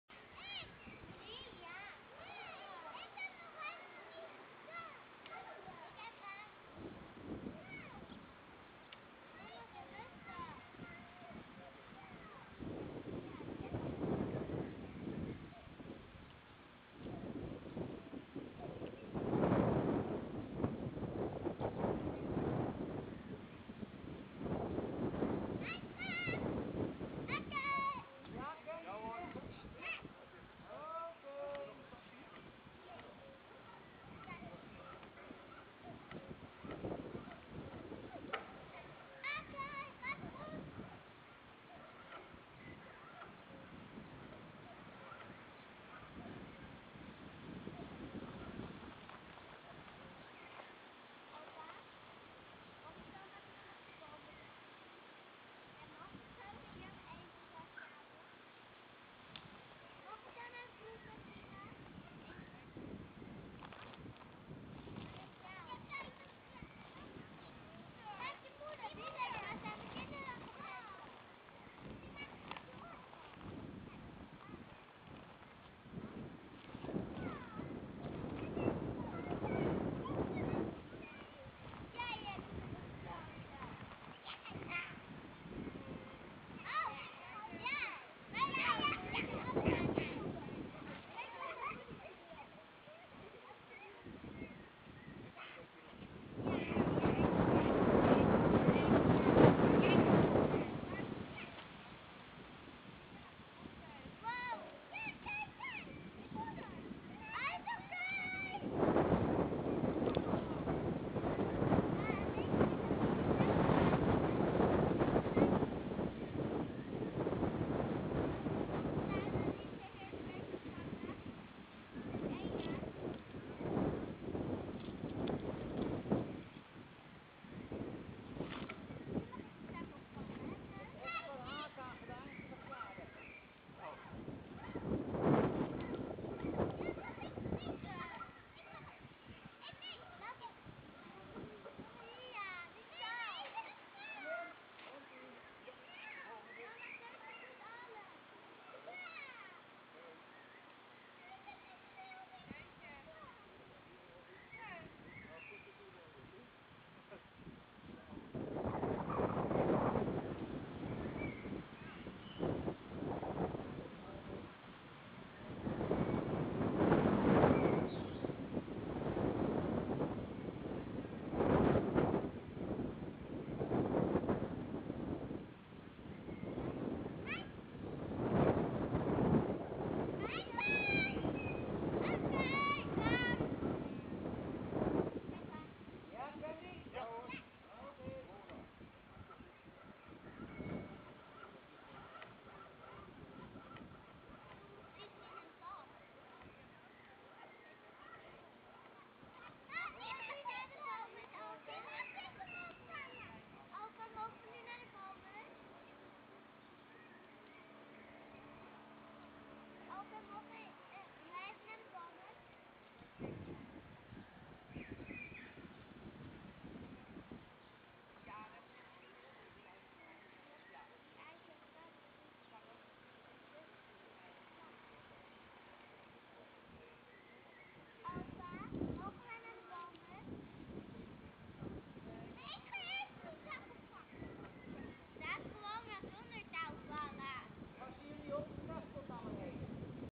Ringen (2) Bigbroeder 2014
Ooievaartjes in Ankeveen  zijn 5 juni  geringd en worden terug gelegd met  klimmer Harry de Rooij die hier in 2004 ook heeft geringd.